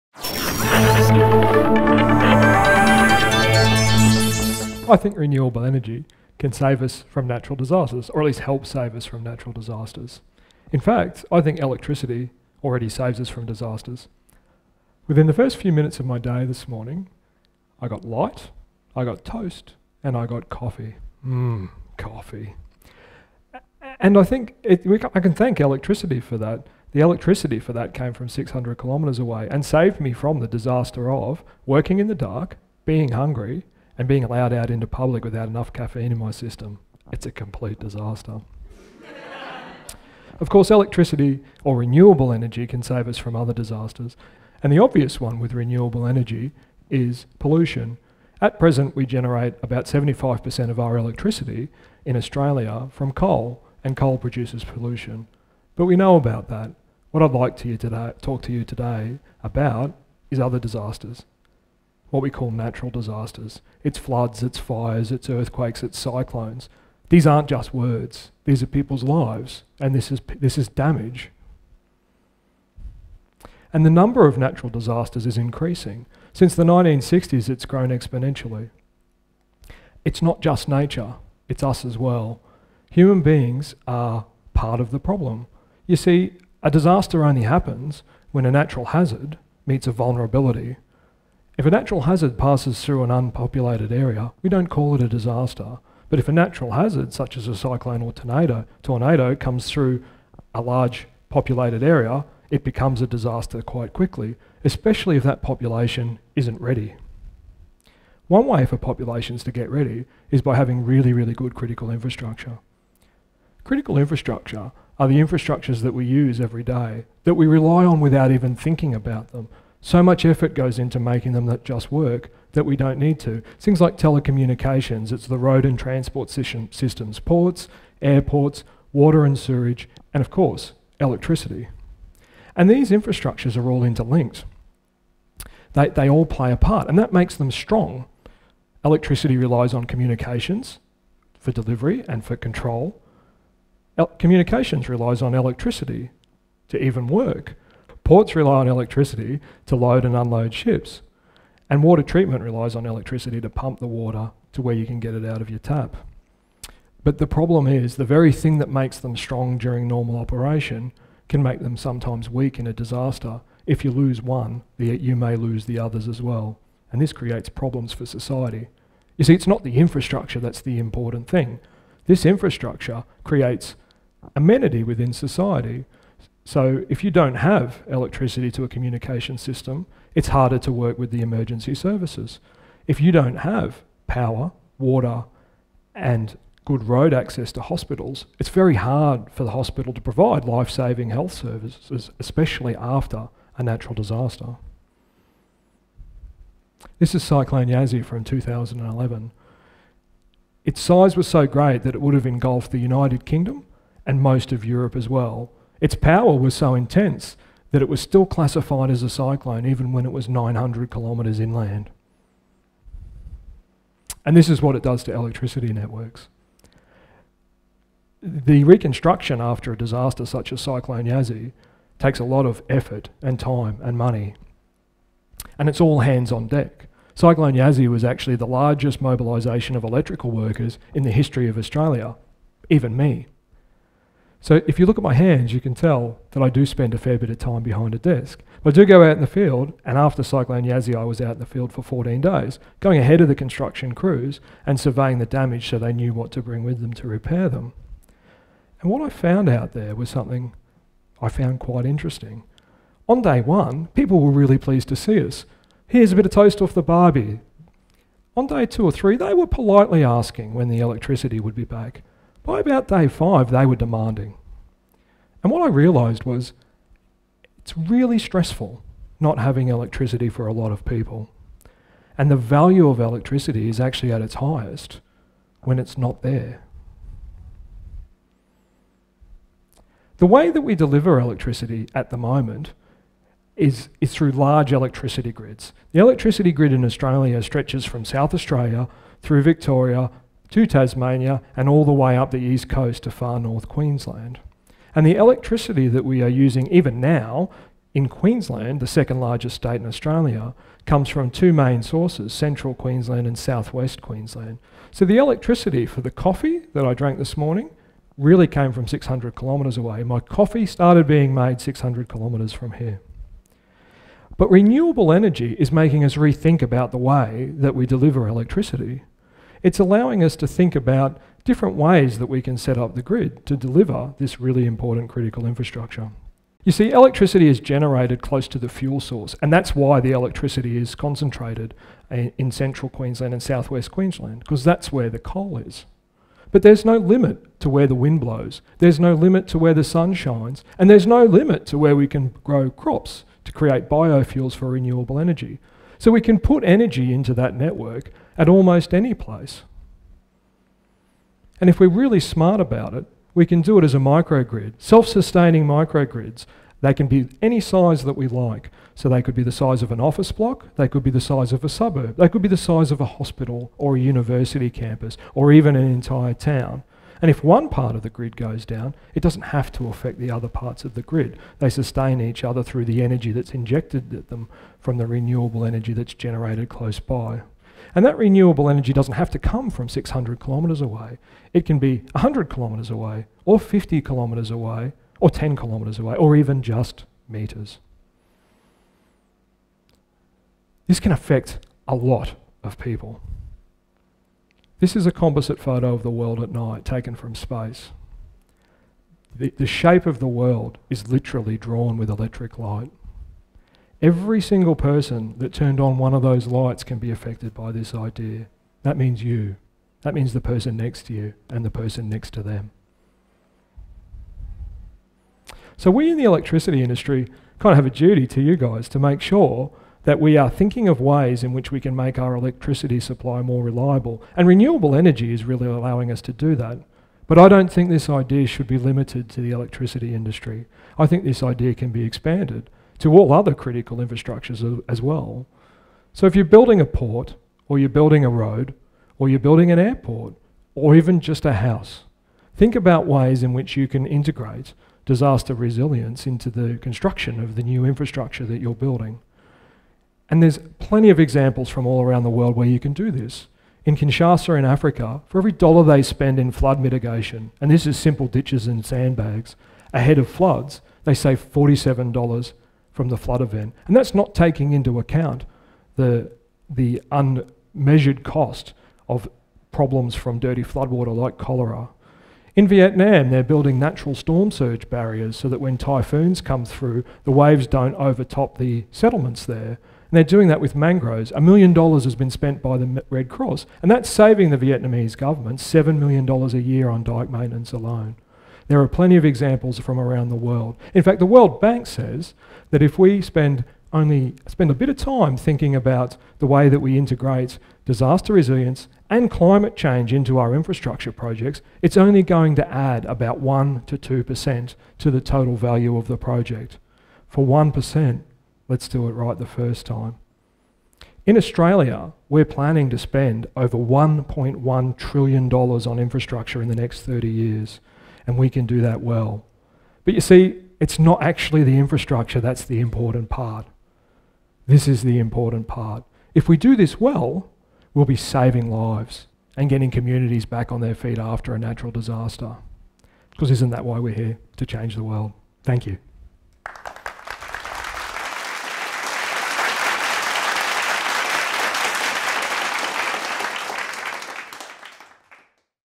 What do renewable energy and natural disasters have in common? | Chris Quin | TEDxTownsville
Worldwide natural disaster impacts are increasing at an alarming rate. The shift to renewable energy invites us to rethink the way we supply electricity. If we re-design the electricity grid and other critical infrastructures with disaster preparedness in mind, we can save lives, help communities withstand natural disasters, and even help save the planet.

Chris Quin is committed to enabling communities to prepare for and recover from natural disasters, and ensuring that critical infrastructures society needs are available rapidly after disaster impact. Chris holds a Masters in Disaster Preparedness and Reconstruction, is a Certified Practicing Project Director with the Australian Institute of Project Management, works as a Project Sponsor for Queensland’s electricity distributor, and is a Deputy Group Leader with the Townsville / Thuringowa State Emergency Service. Chris has responded to emergencies and disasters such as cyclones, severe storms, flooding, search and rescue, bomb evacuations, and getting three young children to school on time.